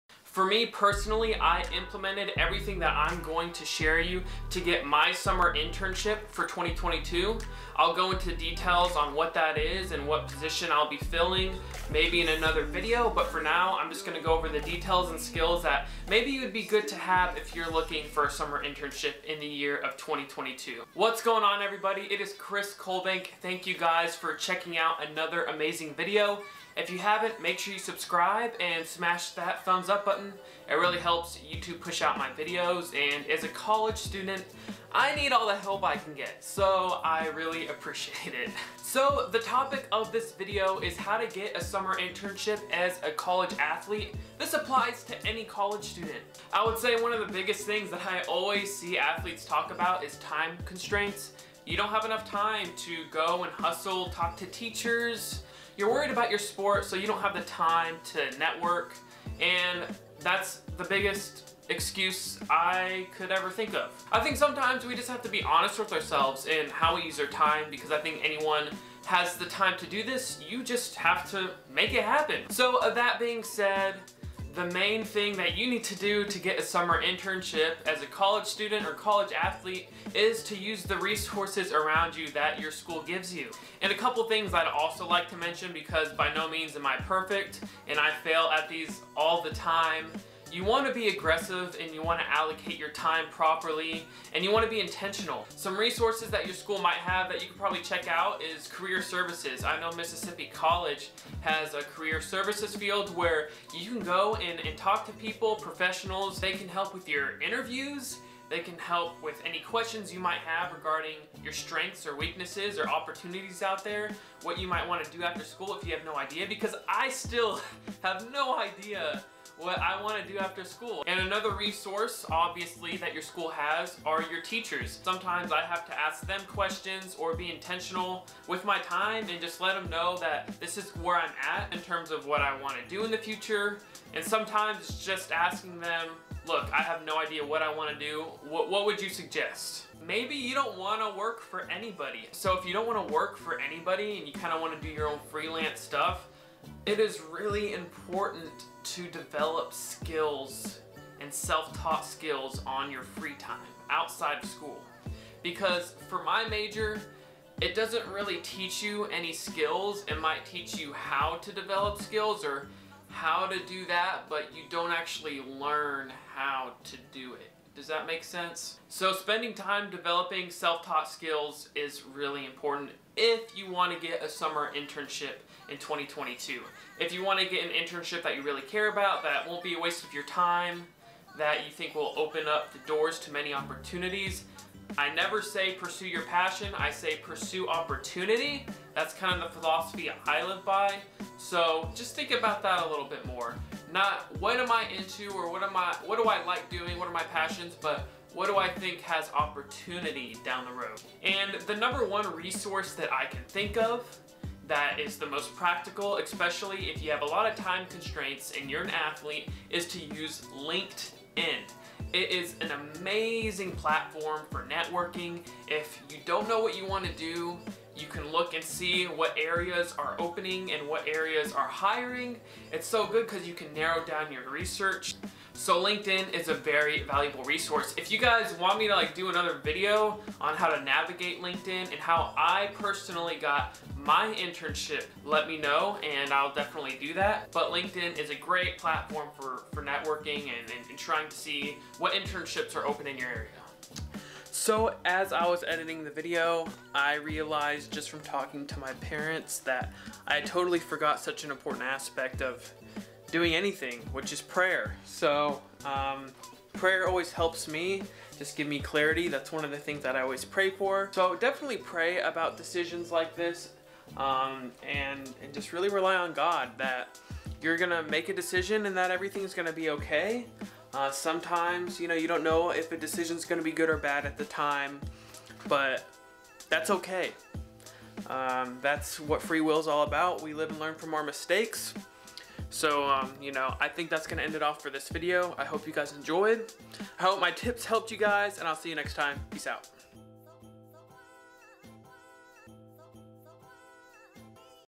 HOW TO GET A SUMMER INTERNSHIP AT THE RIGHT COMPANY! COLLEGE ATHLETE/BUSINESS STUDENT GIVES ADVICE!
These are tricks and resources I used to get an internship at an awesome company I support! I hope this video helps any college student or individual look for jobs! Remember, it is about searching for opportunity, not your passion.